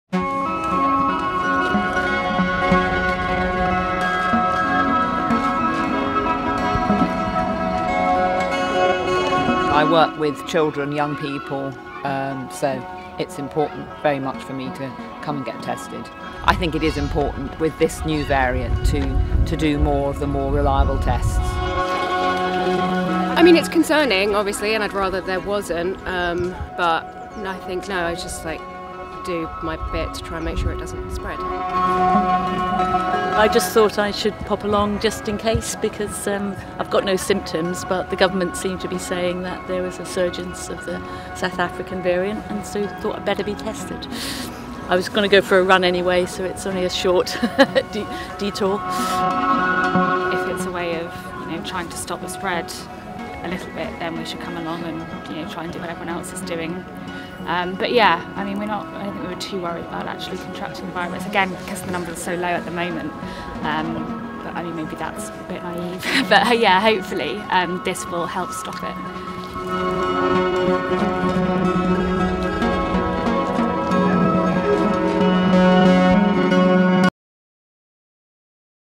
Long queues as Londoners surge test after more South Africa variant detected
Thousands of residents have queued to take coronavirus tests in London following an outbreak of the South Africa variant.